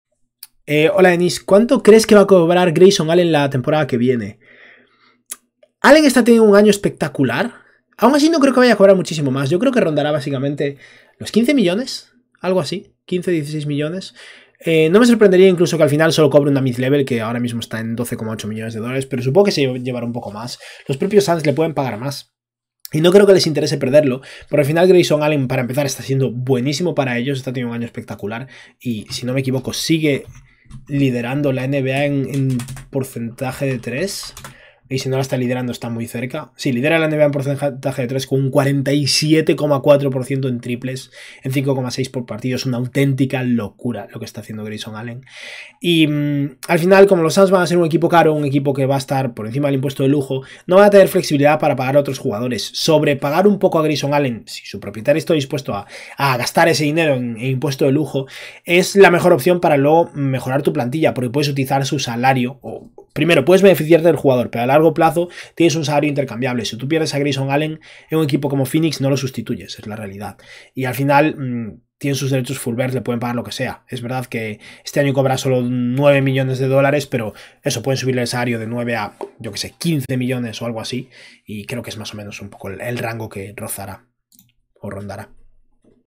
¿Cuál será el mercado de Grayson Allen en la próxima agencia libre? | BASKET 365
Clip en el que intentaremos anticipar cuál será el próximo salario de Grayson Allen, un jugador que ha resultado clave para los Phoenix Suns.